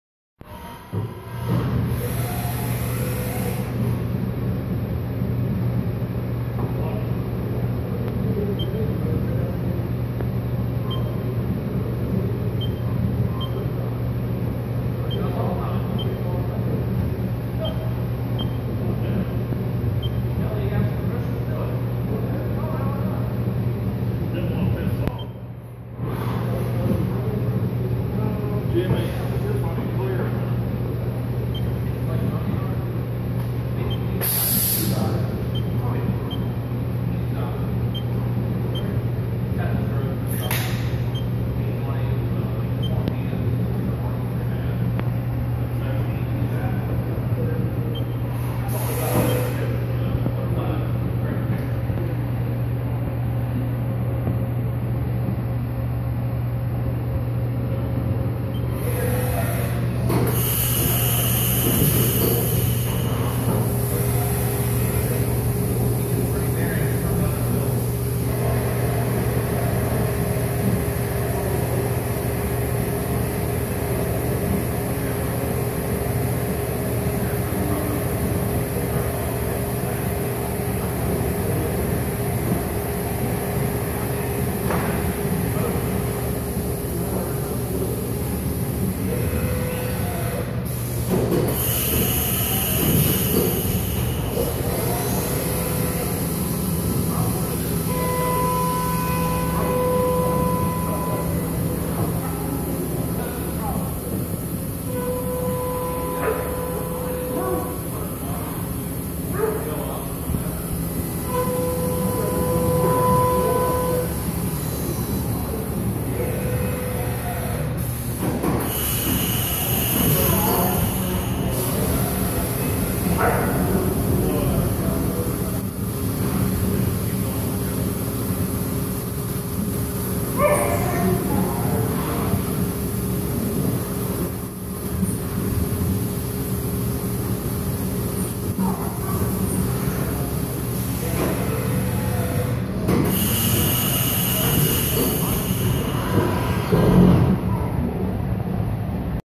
Haas milling probe and tap existing holes
Probing holes Inna rough cast part. Close tolerance investment casting allows direct tapping but the tool must be lined up perfectly. I achieved this using in program probing to set G55 and G56 offsets. There was a relatively close tolerance on those holes to the hole bored in the operation so the program checked the center difference of G54 (center of hole to be bored) with G55 and G56 to assure the casting was within tolerance before any machining took place.